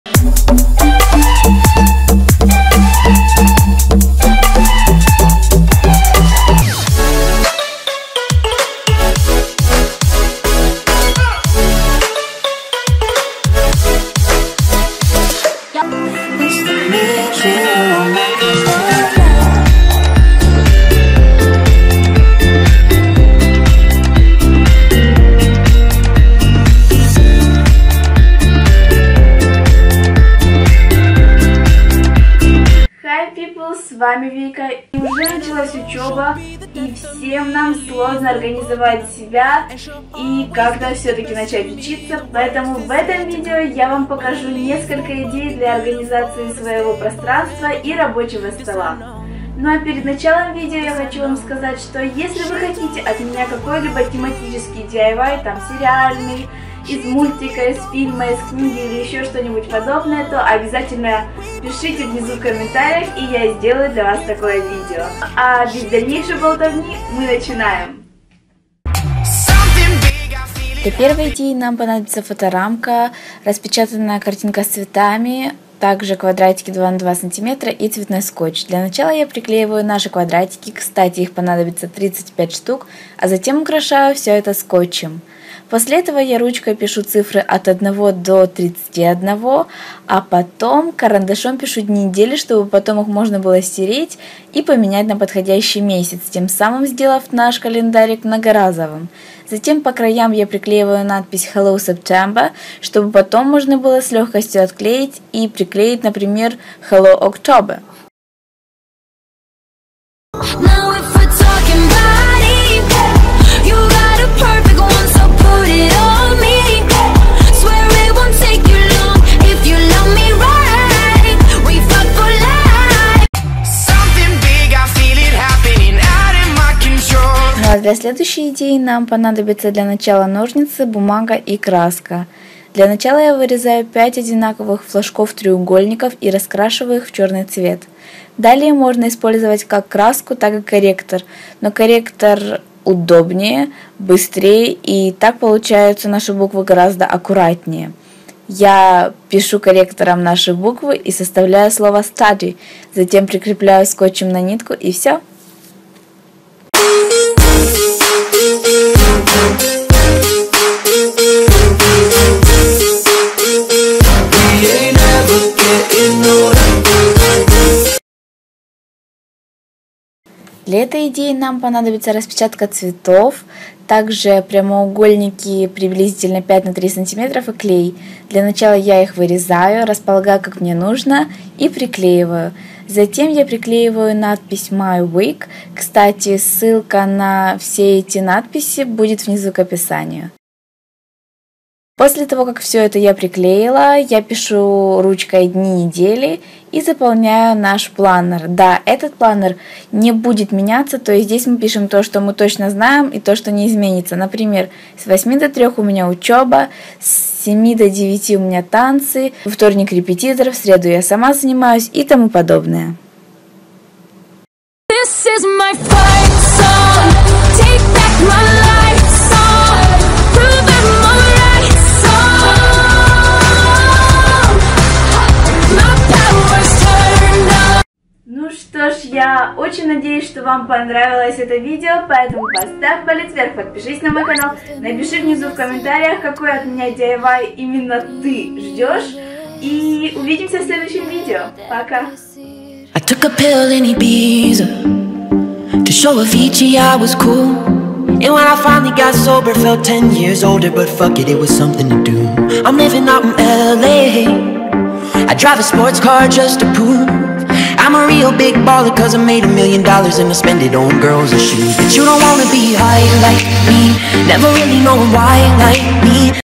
DIY: ОРГАНИЗАЦИЯ РАБОЧЕГО СТОЛА II BACK TO SCHOOL 2016 II
В этом видео я покажу тебе 3 DIY ОРГАНИЗАЦИИ РАБОЧЕГО ПРОСТРАНСТВА ♥

Тег для "вызовов"